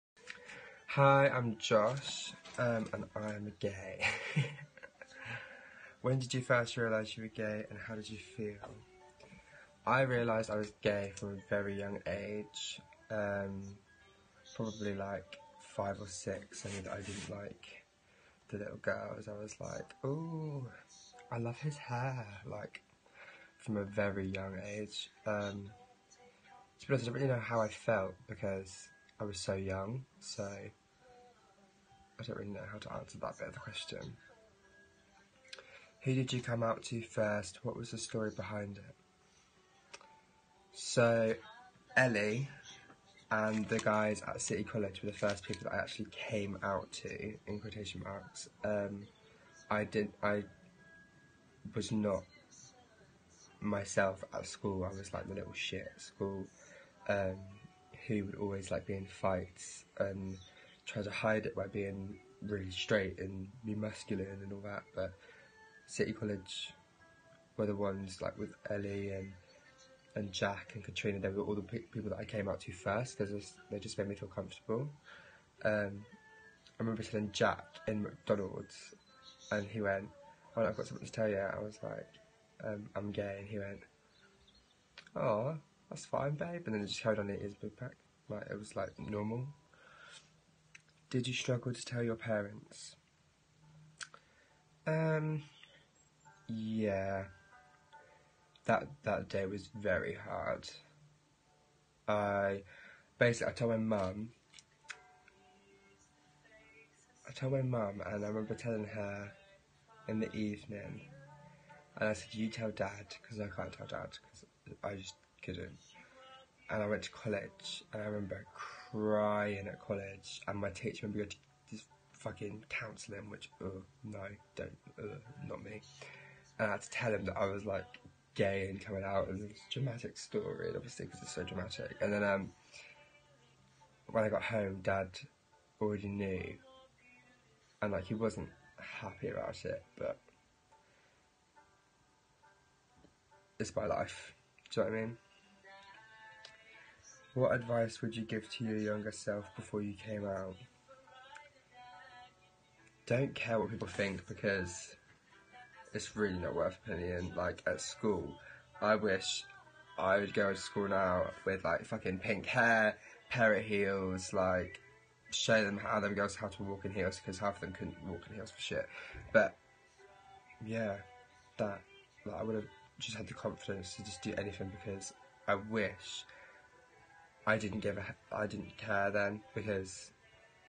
Joshua Jackson in our gay diaries!
Josh took part in 'The Gay Diaries'. This is where people discuss their journey openly and in their own space!